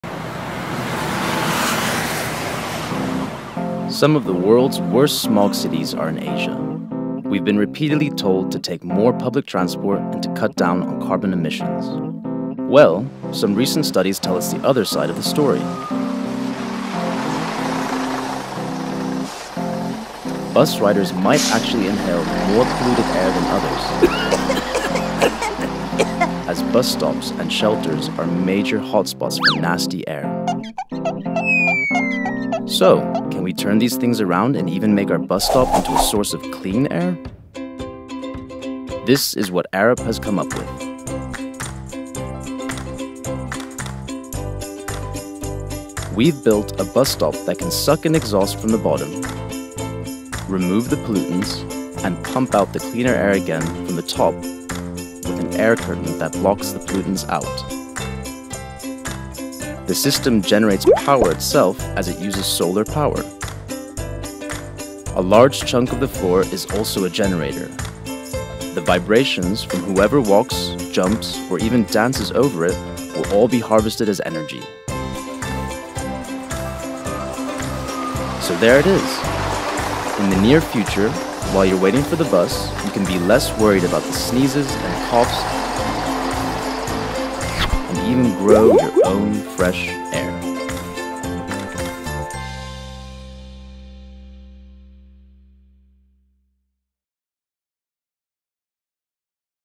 Cleaner city air: A prototype for a roadside air purification system - Arup
Arup and Sino Green jointly developed a prototype of a city air purification system, fabricated in the format of a bus shelter, which can clean air in the open space through filtration.